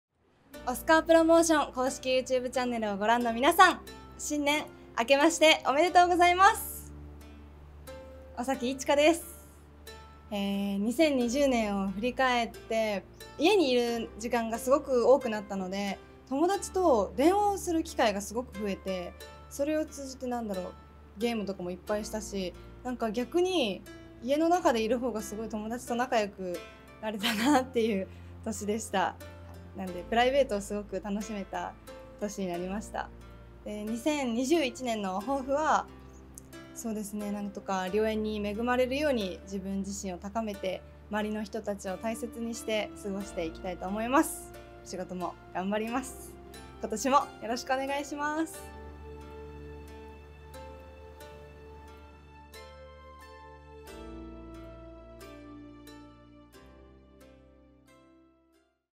【尾碕真花】2021年 新年のご挨拶
尾碕真花の2020年 振り返りと新年の抱負になります！